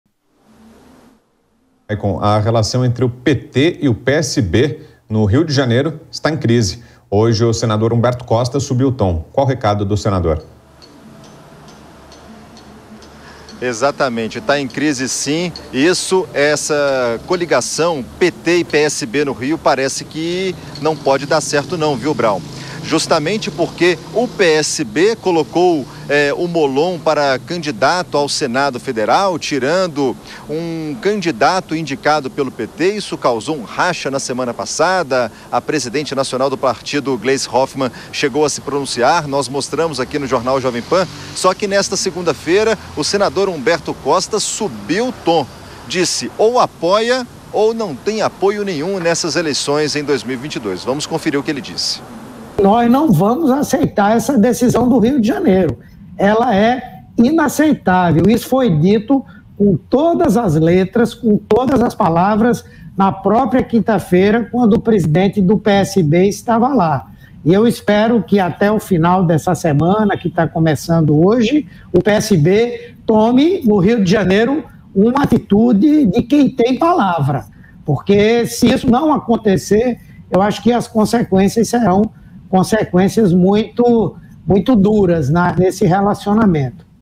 Humberto Costa não aceita quebra de acordo entre PSB e PT
Indicação de Alessandro Molon ao Senado pode desfazer aliança entre partidos no RJ. Humberto Costa: “Espero que até o fim dessa semana o PSB tome uma atitude de quem tem palavra, se isso não acontecer as consequências serão muito duras”.